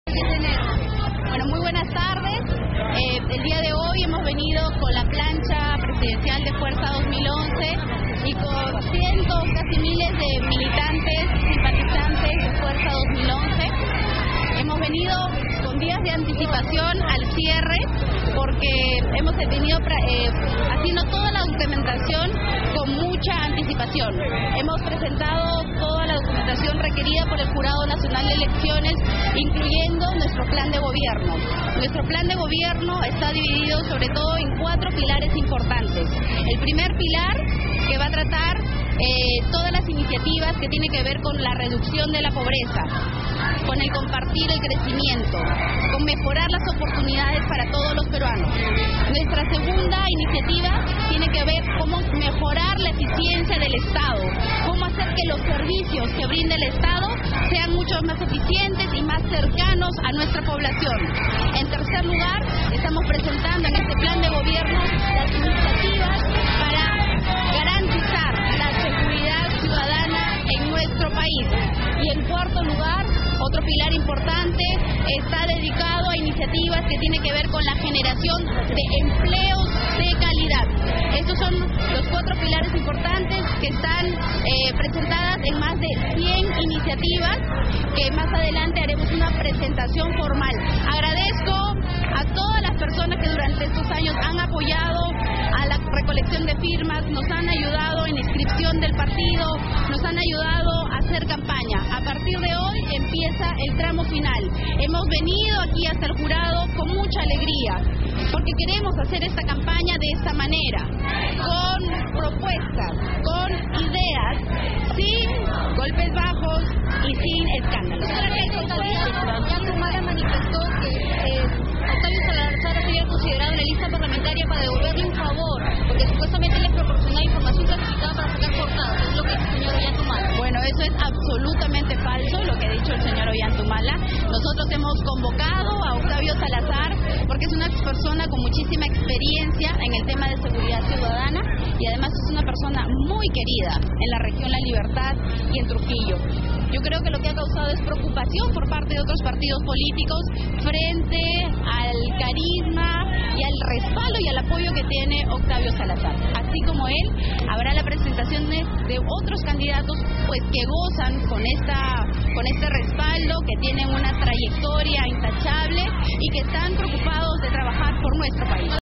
Keiko Fujimori presentó su plancha ante el JNE
Keiko Fujimori presentó su plancha presidencial al JNE.